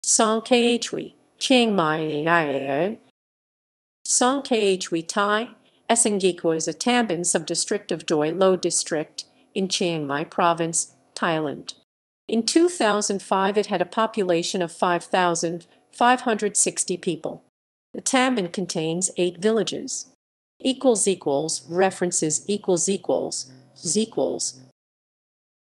Song Khwae, Chiang Mai
Song Khwae (Thai: สองแคว) is a tambon (subdistrict) of Doi Lo District, in Chiang Mai Province, Thailand.  In 2005 it had a population of 5,560 people. The tambon contains eight villages.

== References ==